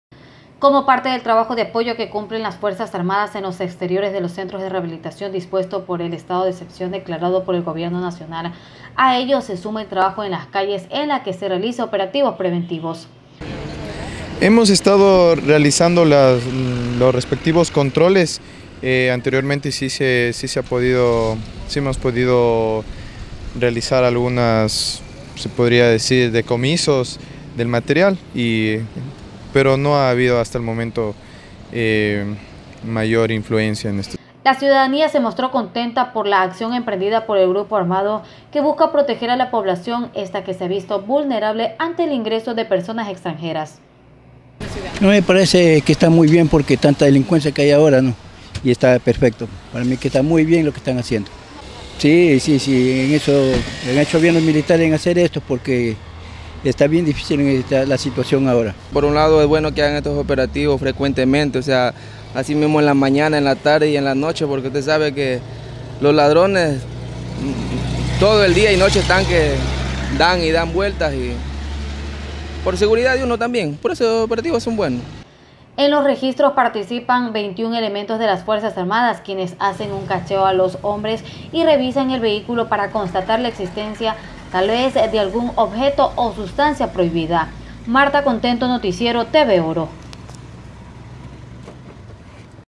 FUERZAS ARMADAS PARTICIPAN EN OPERATIVOS DE CONTROL DE ARMAS Y EXPLOSIVOS
OPERACIONES MILITARES SE CUMPLEN EN EL ORO, CON EL OBJETIVO DE PRECAUTELAR LA SEGURIDAD CIUDADANA LA VERIFICACIÓN Y CONTROL SE ESTA EJECUTANDO EN LUGARES ESTRATÉGICOS.